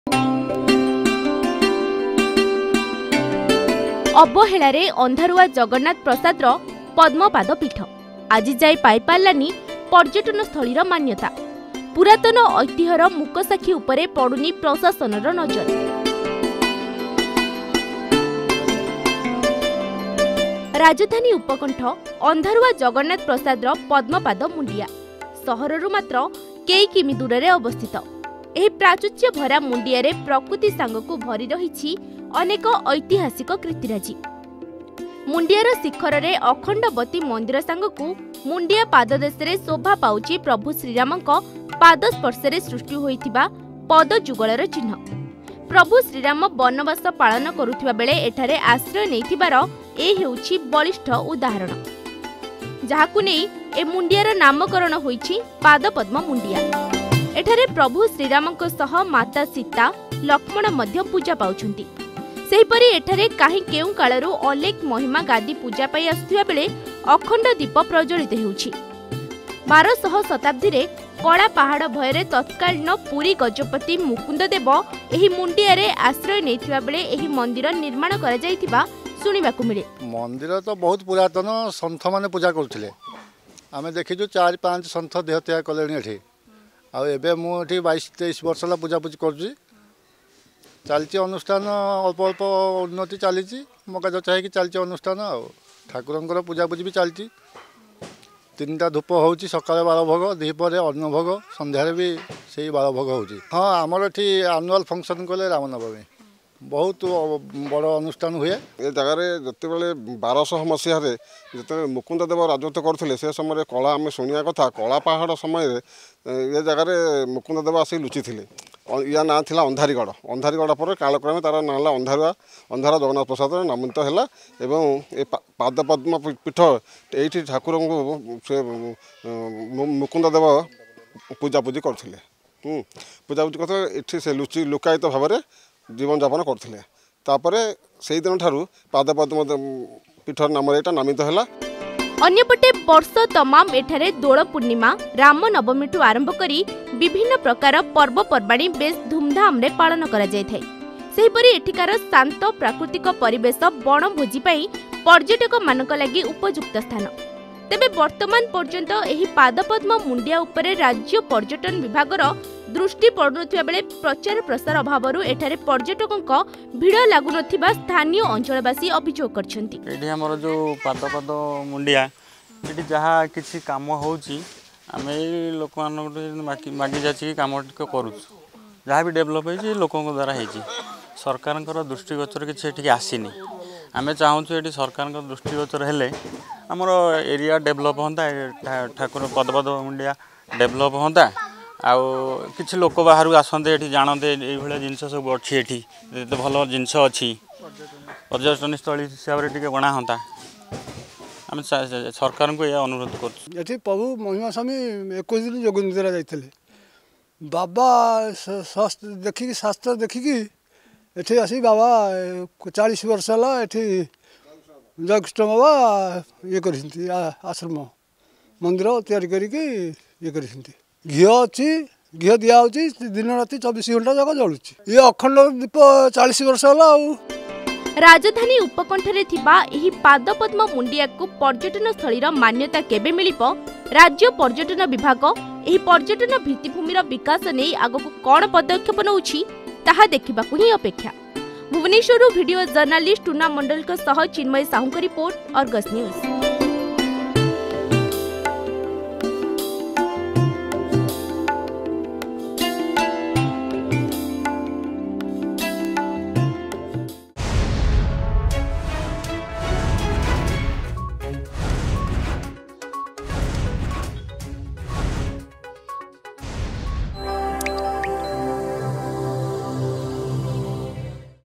ଅନ୍ଧାରରେ ଅନ୍ଧାରୁଆ ପଦ୍ମପାଦ ମୁଣ୍ଡିଆ
ପ୍ରଶାସନର ନଜର ଆଢୁଆଳରେ ଅନ୍ଧାରୁଆ ଜଗନ୍ନାଥ ପ୍ରସାଦର ପଦ୍ମପାଦ ମୁଣ୍ଡିଆ । ରାଜଧାନୀ ଉପକଣ୍ଠରେ ଥିବା ଏହି ସ୍ଥାନରେ ରହିଛି ଐତିହାସିକ କାର୍ତ୍ତିରାଜି । ଏହା ସହ ପ୍ରାକୃତିକ ସୌନ୍ଦର୍ଯ୍ୟ ସହ ଏଠାରେ ଶ୍ରୀରାମଙ୍କ ପଦଯୁଗଳର ଚିହ୍ନ ଶୋଭାପାଉଛି.